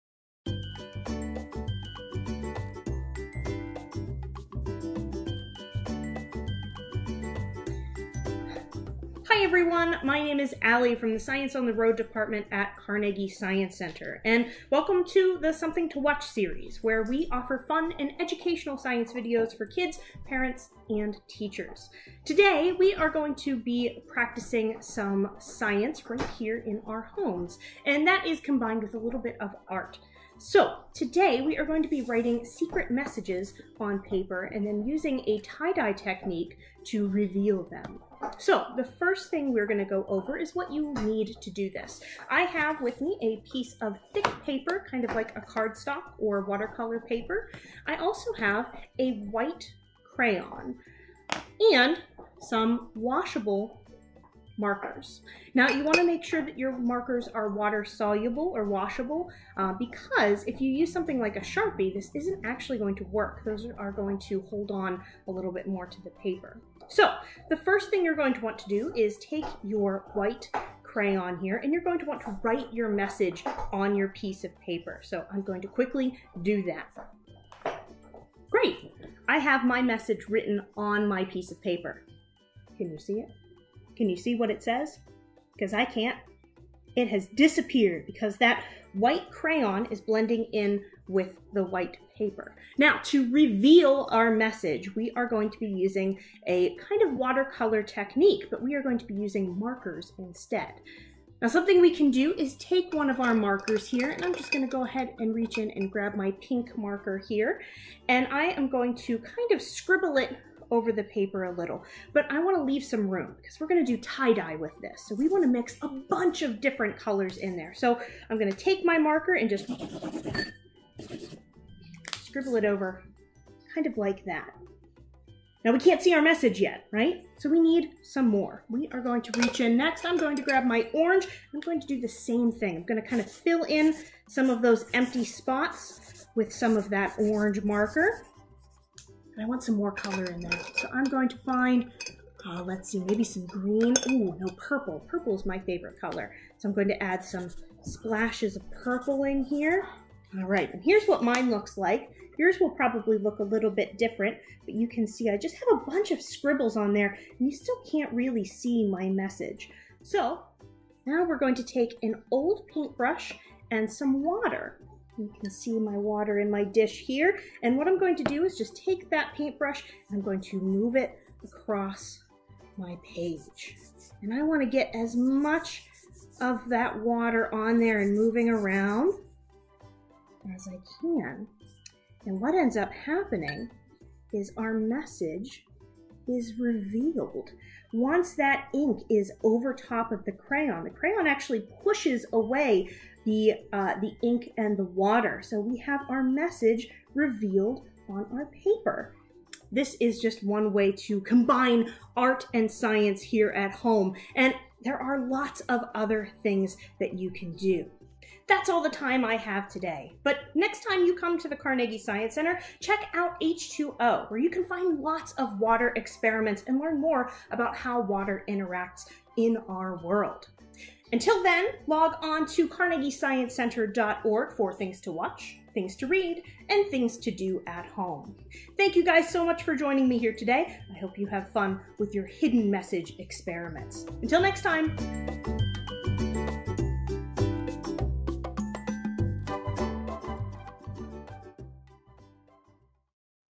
Tie-Dye Secret Messages with Carnegie Science Center
Something to WATCH: Science Educator Aly Toy shows you how to make tie-dye secret messages using paper, a white crayon, washable markers, a paintbrush, and water.

Presented by Highmark Blue Cross Blue Shield